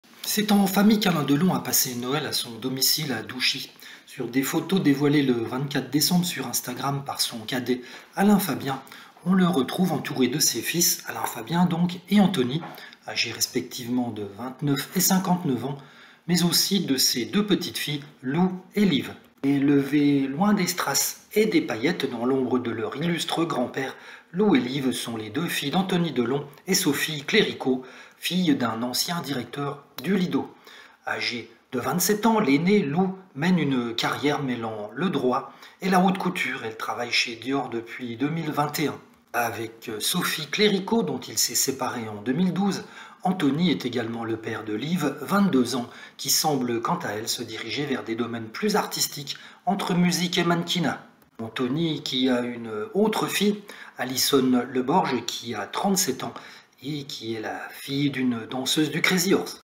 Qui sont Lou et Liv, les deux petites-filles d’Alain Delon, à ses côtés le soir de Noël ?
Dimanche 24 décembre, Alain Delon a passé son réveillon de Noël, dans sa demeure de Douchy, dans le Loiret. Il était, pour l’occasion, entouré de ses fils, Alain-Fabien et Anthony, ainsi que de ses deux petites-filles Liv et Loup. Anouchka manquait à l'appel, elle était à l'hôpital